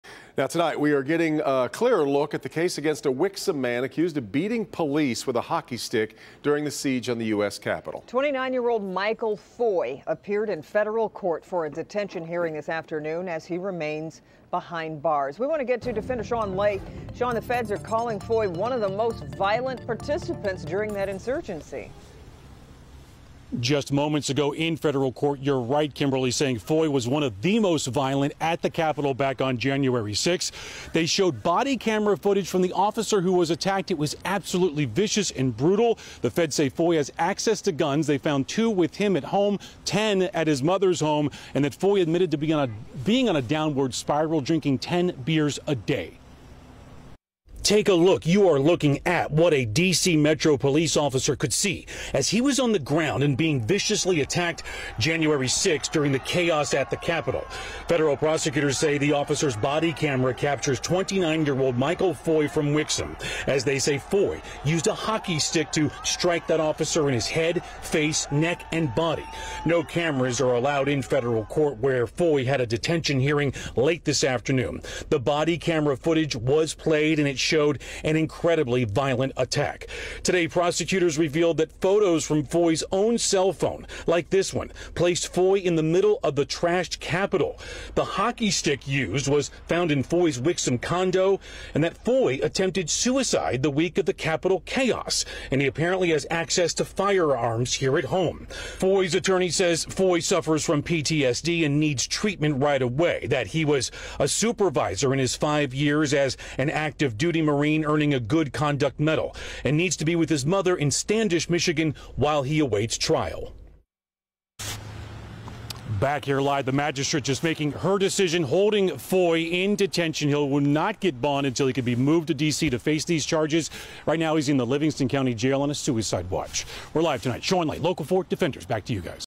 Feds: Wixom man was one of the most violent participants in siege on US Captiol
A Wixom man was arrested after video showed him attacking a police officer with a hockey stick during the US Captiol riot.